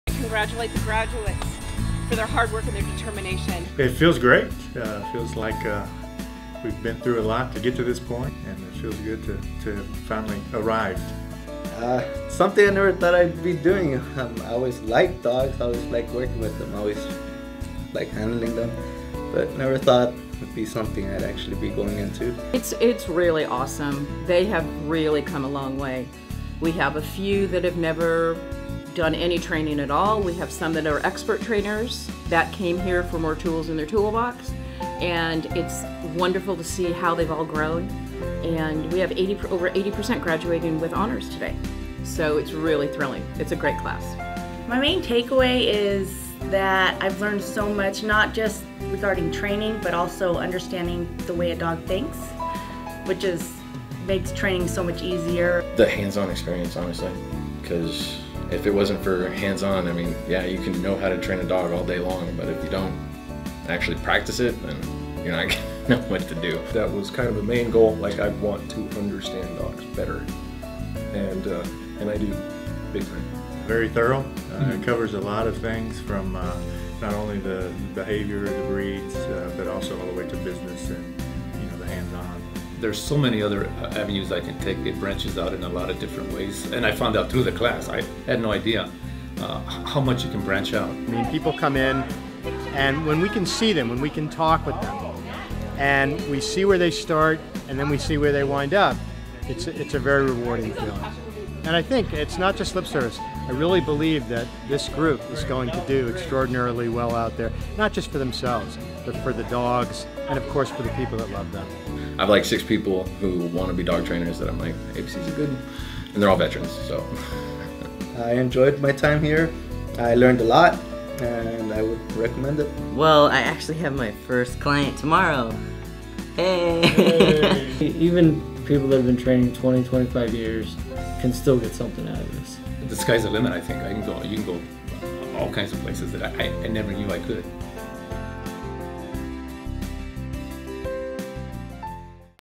ABC Dog Trainers Class of 2016
Animal Behavior College would like to congratulate Class of 2016! These graduates spent five months learning about dog behavior, dog training techniques, and business building tips. As ABC certified dog trainers (ABCDT), they can go out and help the dogs in their local communities.

If you'd like to enroll in our on-site Dog Obedience Instructor Training Program, please